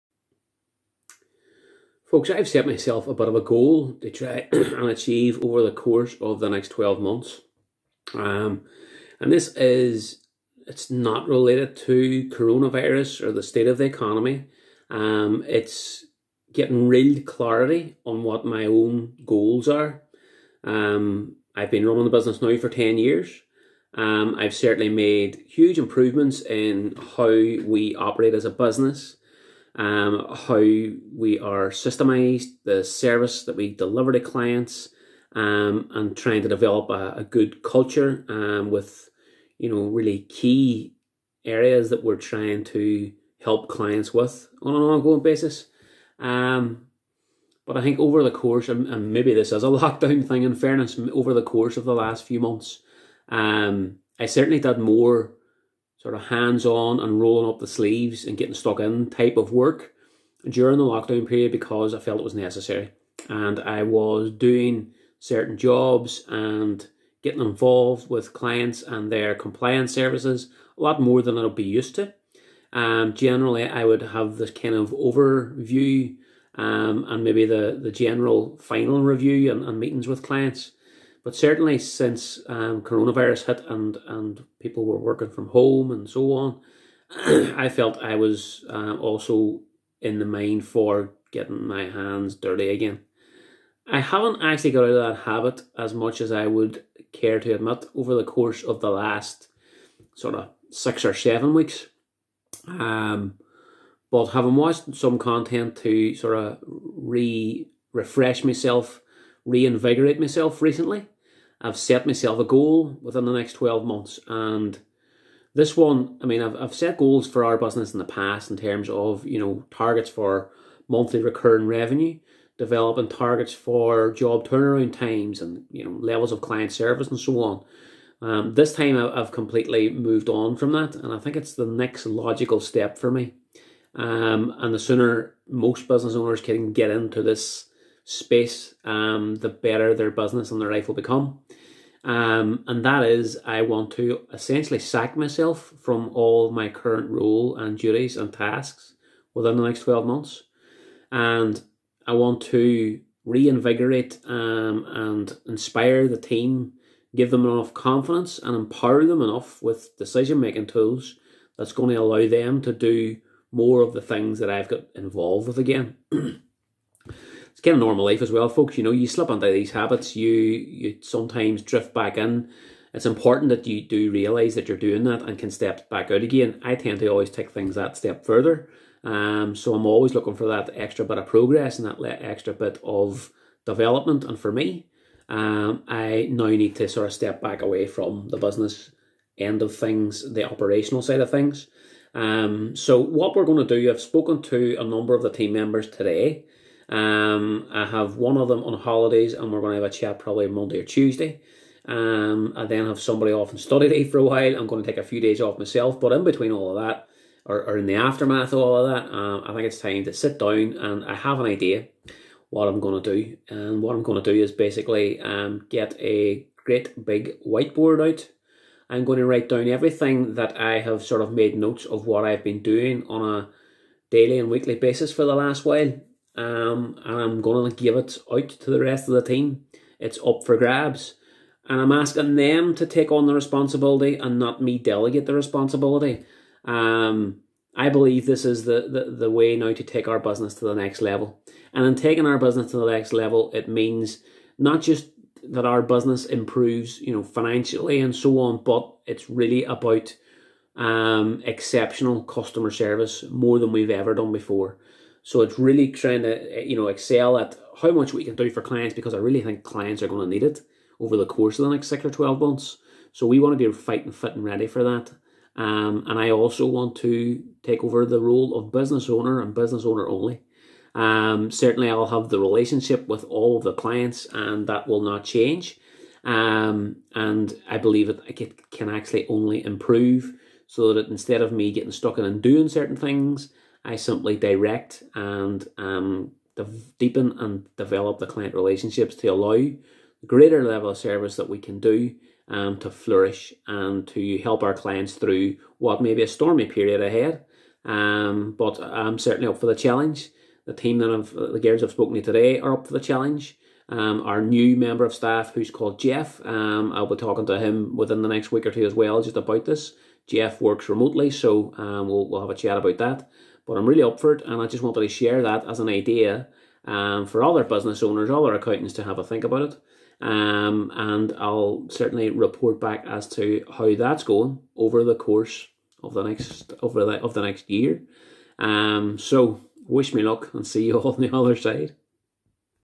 The time has come to change the future
This one is a more personal video. Talking about my own business goals for the next 12 months.

Hopefully other business owners can take something from this, maybe take a similar path.

I've been taking in more and more content from two people I follow online. James Sinclair and my friend James Ashford.

Here's to making it happen!